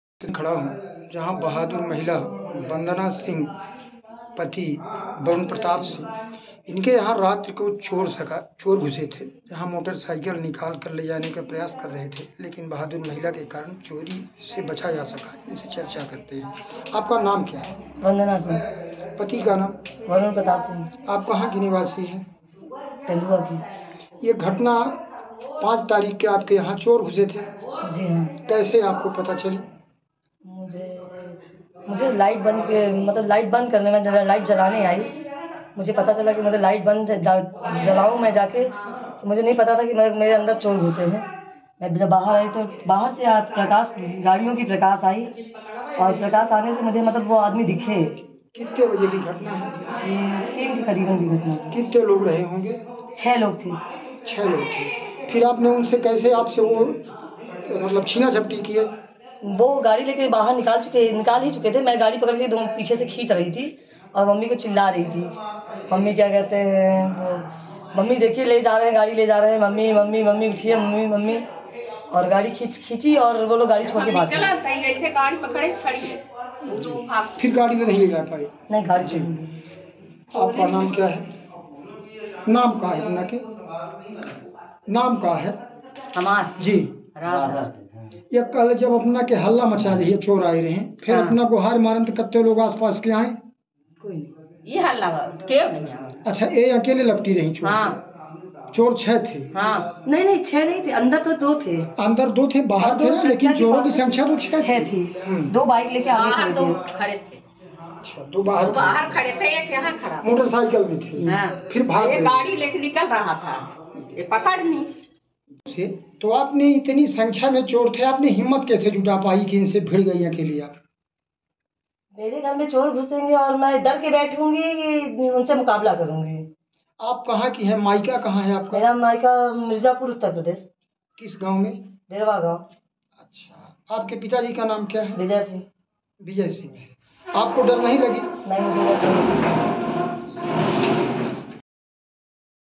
Reva : महिला की हिम्मत के सामने पस्त हुए चोर || Newstrack | Apna Bharat TV
Thieves, beaten in front of the courage of the woman, ran away after leaving the stolen goods